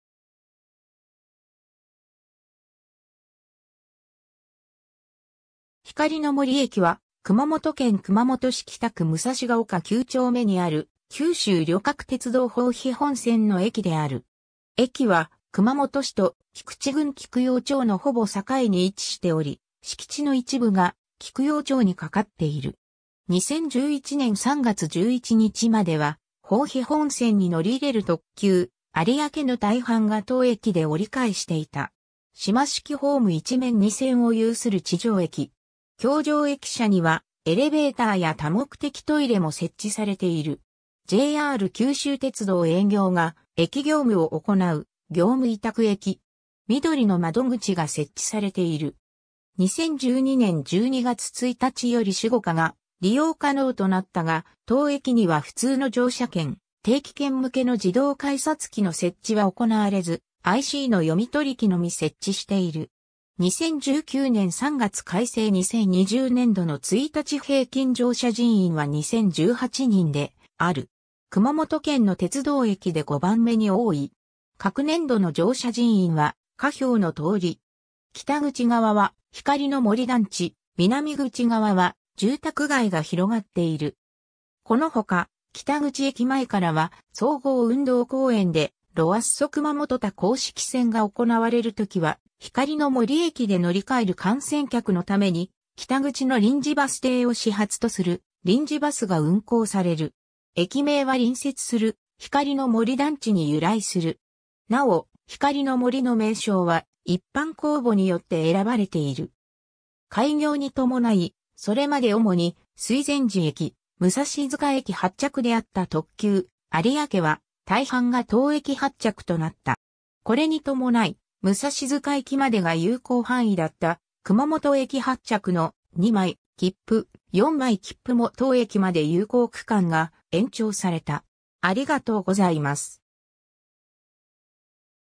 光の森駅
光の森駅（ひかりのもりえき）は、熊本県熊本市北区武蔵ケ丘九丁目にある、九州旅客鉄道（JR九州）豊肥本線の駅である。
 駅は熊本市と菊池郡菊陽町のほぼ境に位置しており、敷地の一部が菊陽町に掛かっている。
2011年3月11日までは、豊肥本線に乗り入れる特急「有明」の大半が当駅で折り返していた。
橋上駅舎にはエレベーターや多目的トイレも設置されている。
 JR九州鉄道営業が駅業務を行う業務委託駅。
みどりの窓口が設置されている。
2012年12月1日よりSUGOCAが利用可能となったが、当駅には普通の乗車券・定期券向けの自動改札機の設置は行われず、ICの読み取り機のみ設置している。
 2019年3月改正 2020年度の1日平均乗車人員は2,018人である。
熊本県の鉄道駅で5番目に多い。
 各年度の乗車人員は下表の通り。
 北口側は光の森団地、南口側は住宅街が広がっている。
 このほか、北口駅前からは、総合運動公園でロアッソ熊本他公式戦が行われるときは、光の森駅で乗り換える観戦客のために北口の臨時バス停を始発とする臨時バスが運行される。
 駅名は隣接する光の森団地（菊陽町）に由来する。
なお、「光の森」の名称は一般公募によって選ばれている。
 開業に伴い、それまで主に水前寺駅・武蔵塚駅発着であった特急「有明」は大半が当駅発着となった。
これに伴い、武蔵塚駅までが有効範囲だった熊本駅発着の「2枚きっぷ・4枚きっぷ」も当駅まで有効区間が延長された。